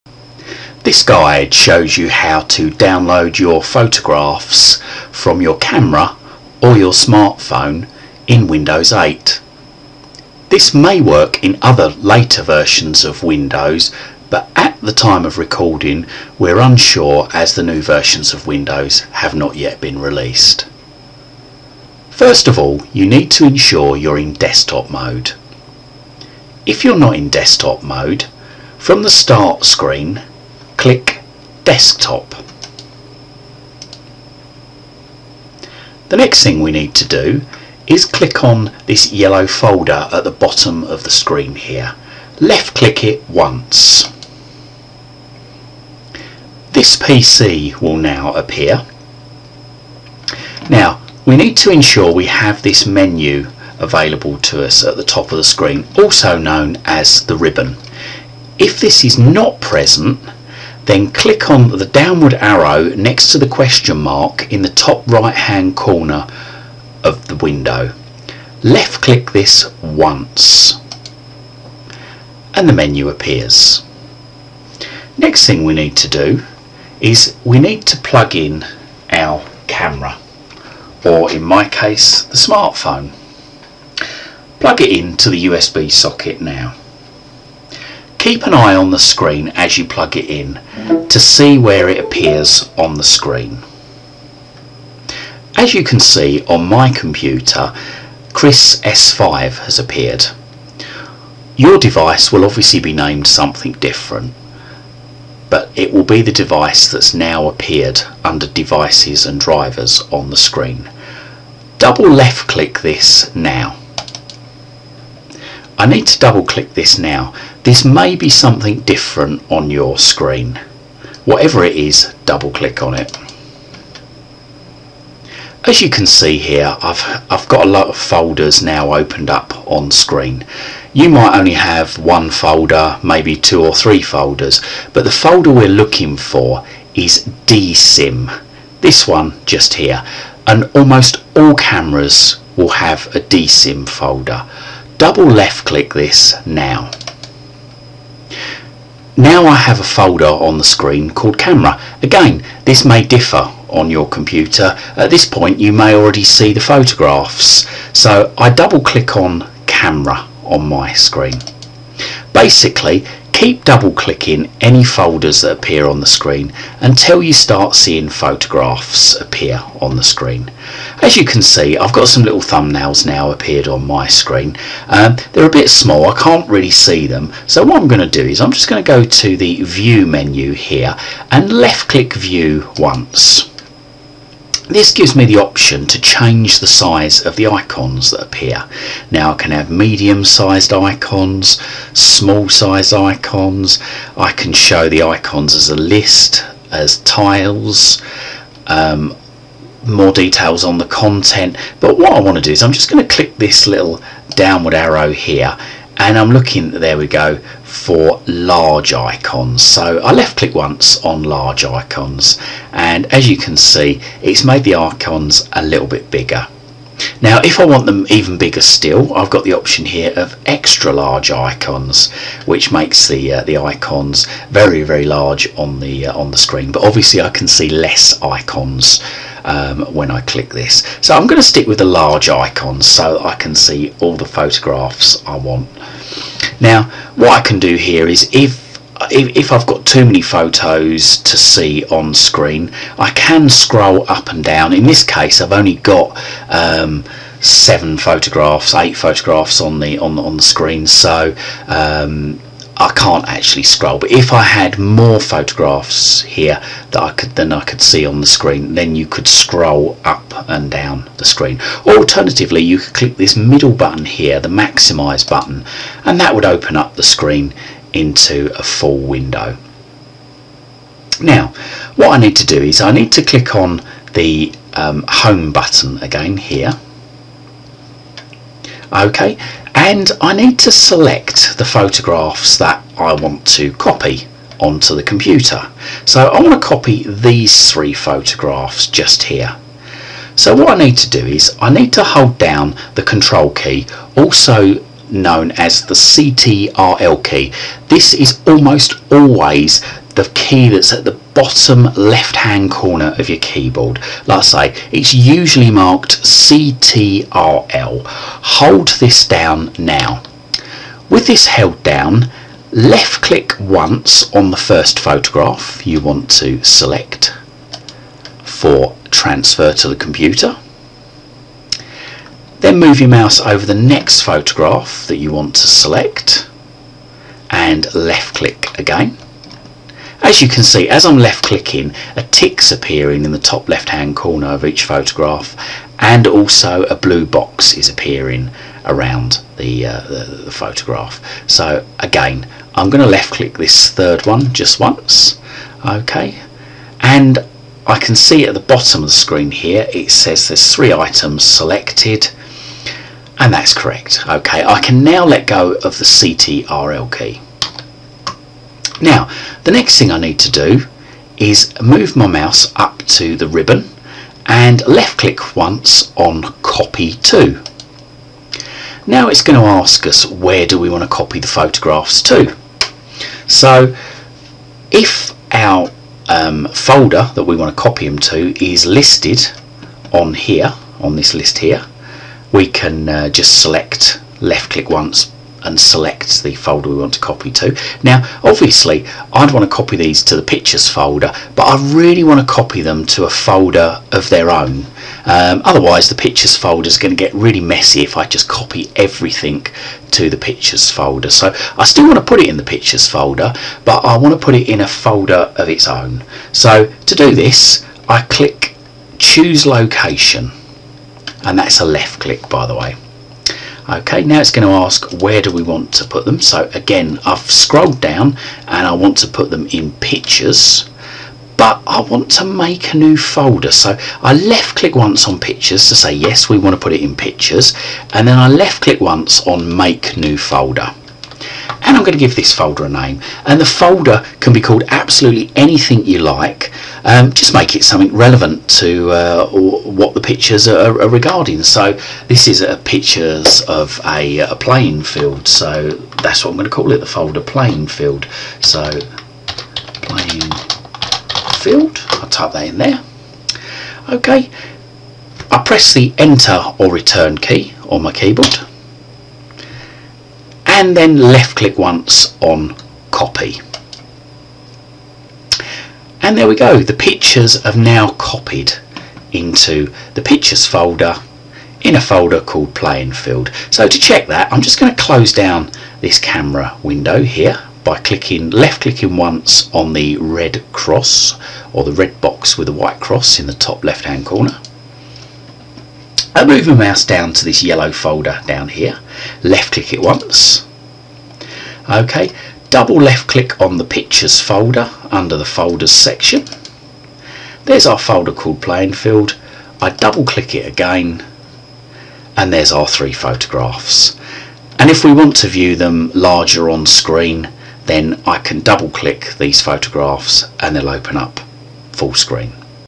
Download Pictures from your Camera or Phone to Windows 8
How to manually download pictures from your camera or smartphone in Windows 8 and Windows 8.1